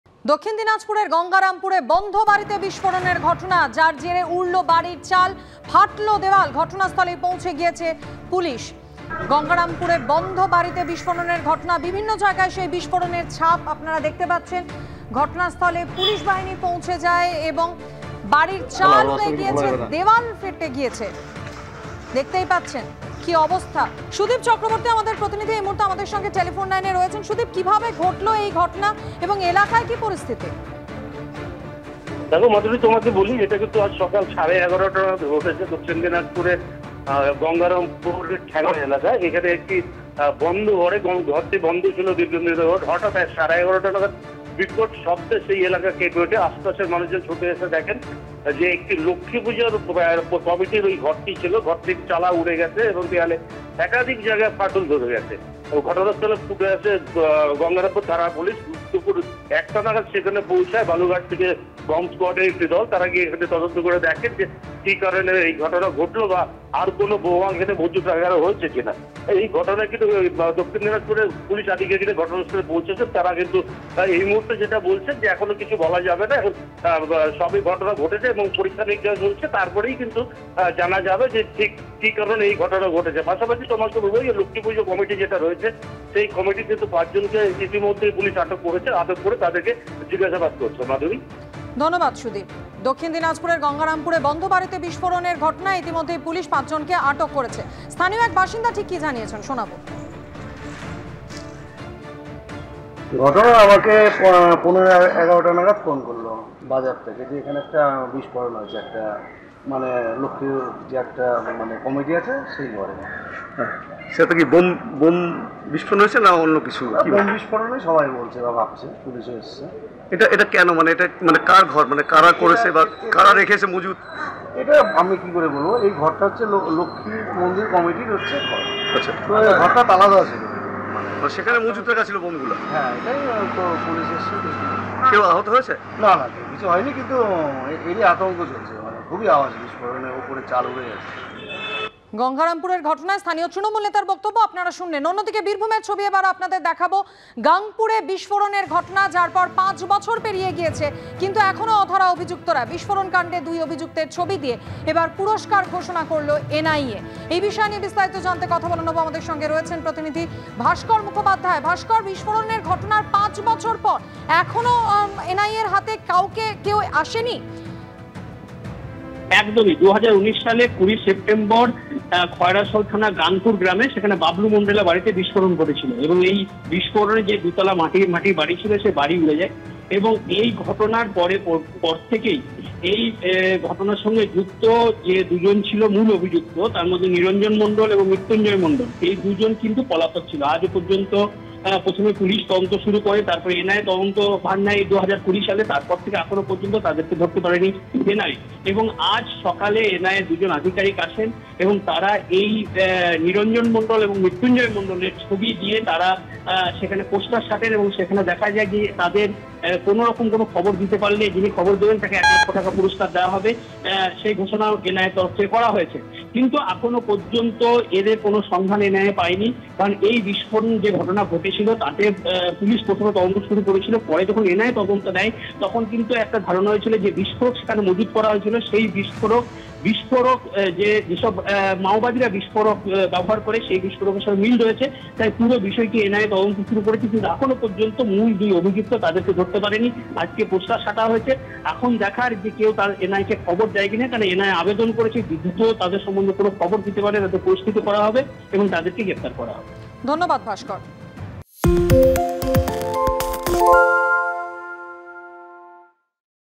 South Dinajpur: গঙ্গারামপুরে বন্ধ বাড়িতে বিস্ফোরণ, উড়ল বাড়ির চাল, ফাটল দেওয়াল | ABP Ananda LIVE
ABP Ananda LIVE: দক্ষিণ দিনাজপুরের গঙ্গারামপুরে বন্ধ বাড়িতে বিস্ফোরণ। উড়ল বাড়ির চাল, ফাটল দেওয়াল। ঘটনাস্থলে পুলিশ, আটক ৫